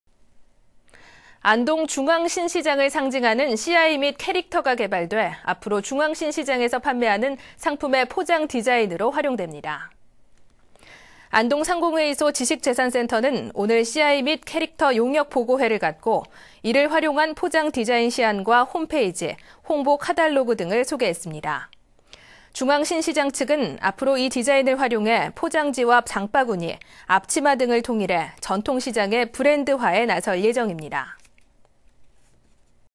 [안동MBC뉴스] 안동 중앙신시장, CI.캐릭터 개발
2013/10/11 09:07:22    작성자 : 정윤호

 안동 중앙신시장을 상징하는 CI 및 캐릭터가
개발돼, 앞으로 중앙신시장에서 판매하는
상품의 포장디자인으로 활용됩니다.
         ◀ＥＮＤ▶

 안동상공회의소 지식재산센터는 오늘
CI 및 캐릭터 용역 보고회를 갖고,
이를 활용한 포장디자인 시안과, 홈페이지,
홍보 카탈로그 등을 소개했습니다.

 중앙신시장측은 앞으로 이 디자인을 활용해
포장지와 장바구니, 앞치마 등을 통일해
전통시장의 브랜드화에 나설 예정입니다.